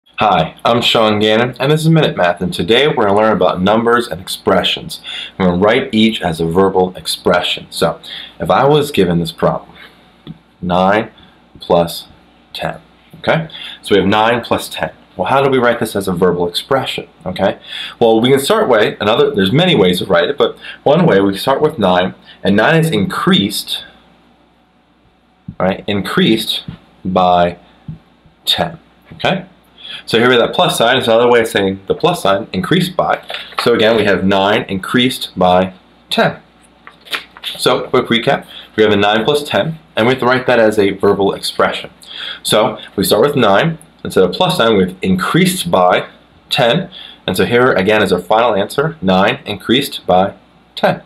Write as a Verbal Expression 9+10
A math video lesson on Writing Algebraic Expressions as Verbal Expressions. In this video, we write 9+10 as a verbal expression